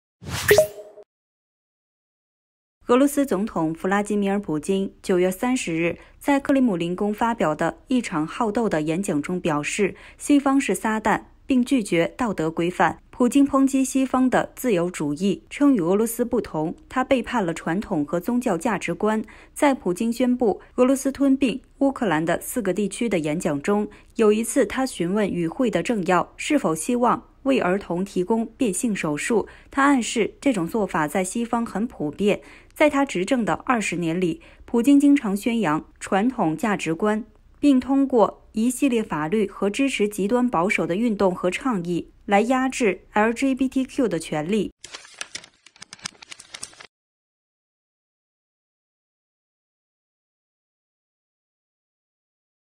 普京抨击西方“邪恶”，称赞“传统”俄罗斯价值观
俄罗斯总统弗拉基米尔·普京周五（9 月 30 日）在克里姆林宫发表的一场好斗的演讲中表示，西方是“撒旦”并拒绝“道德规范”。普京抨击西方的自由主义，称与俄罗斯不同，它背离了“传统”和“宗教”价值观。在普京宣布俄罗斯吞并乌克兰的四个地区的演讲中，有一次他询问与会的政要是否希望“为儿童提供变性手术”，他暗示这种做法在西方很普遍。在他执政的二十年里，普京经常宣扬他所说的“传统价值观”，并通过一系列法律和支持极端保守的运动和倡议来压制 LGBTQ 的权利。